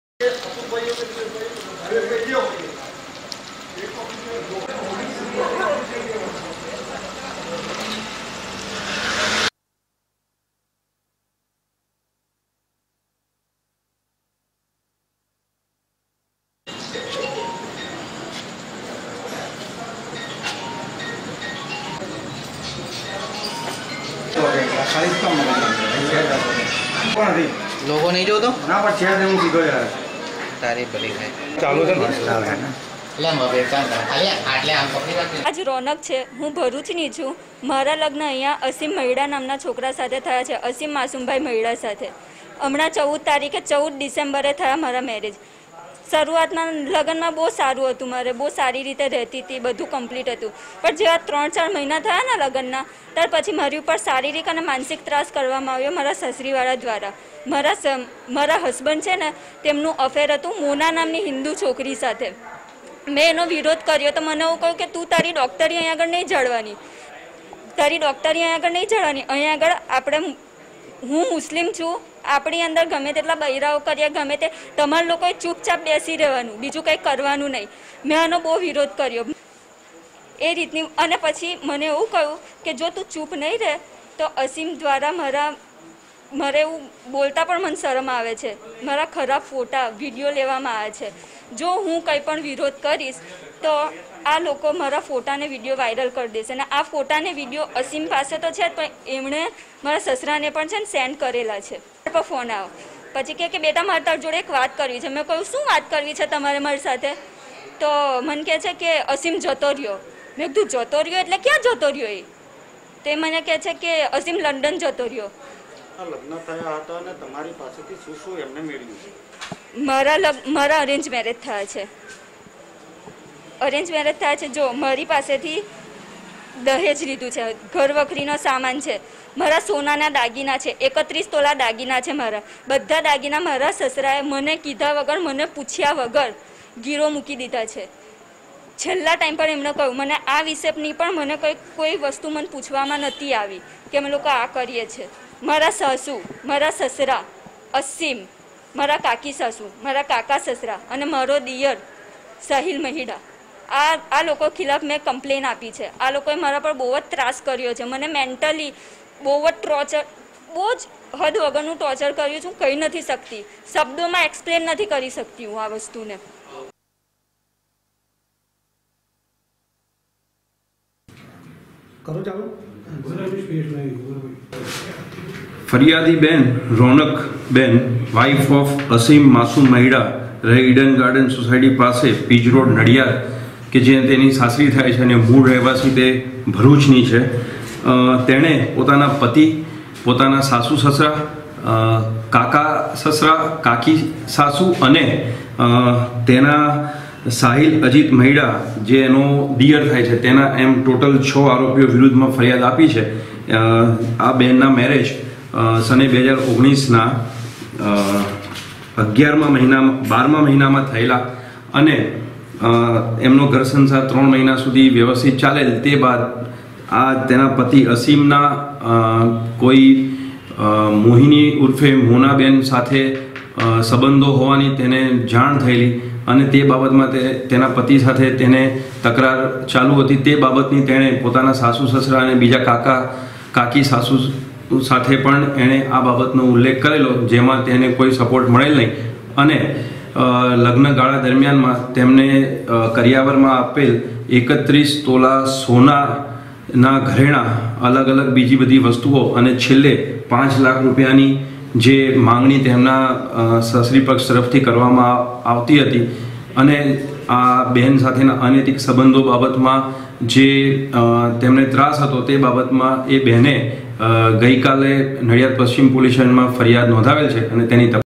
નડિયાદ : પુત્રવધૂને તેના સાસરિયાઓ ત્રાસ આપતા હોવાની ફરિયાદ
નડિયાદના અસીમ માસુમ મહિડા (મોરેસલામ ગરાસિયા) ની પત્નીએ તેના પતિ, સાસુ, સસરા સહીત પરિવારના છ સભ્યો વિરુદ્ધ નોંધાવી ફરિયાદ।. મહિલાને તેના પતિ અને સાસરિયા દ્વારા શારીરિક માનસિક ત્રાસ અપાતો હોઈ, તેનો પતિ અન્ય સ્ત્રી સાથે સંબંધ ધરાવતો હોઈ અને તેના 31 તોલા દાગીના તેના સસરા દ્વારા તેની જાણ બહાર ગીરવે મૂકી દઈ, તેનો પતિ તેને મૂકી તેને કહ્યા વગર લંડન જતો રહેતા આ મામલે તેને તમામ વિરુદ્ધ નડિયાદ પશ્ચિમ પોલીસ મથકે ફરિયાદ નોંધાવી છે.